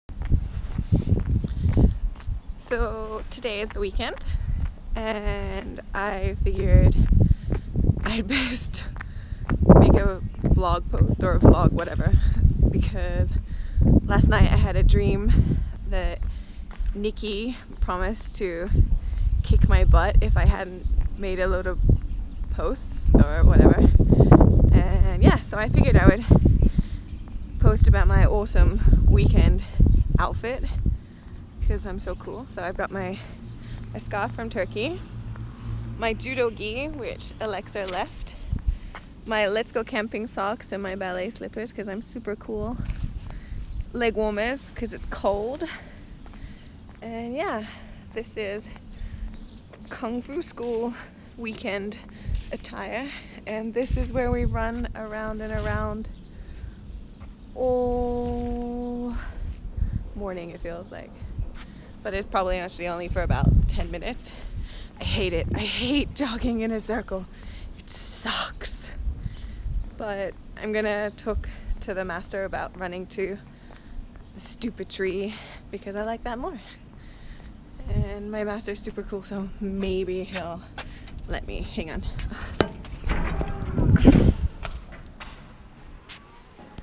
Kung Fu Fashion
What should you wear at Kung Fu school in China during the winter?

The easy answer... anything and everything from the free shelf.

A good portion of what I wore during my time in at the school in China I wore things from the free shelf. It was freezing all the time and I hadn't been able to pack as many winter things as I wanted and I just assumed I would be able to buy some. However living in the middle of nowhere made this difficult and it turns out I was poorer than I expected. So as long as you are warm and comfortable, rock any style you want at Kung Fu school or wherever you may be.

One of many random clips from my time in China I found on a memory card I had long thought lost to a "safe place".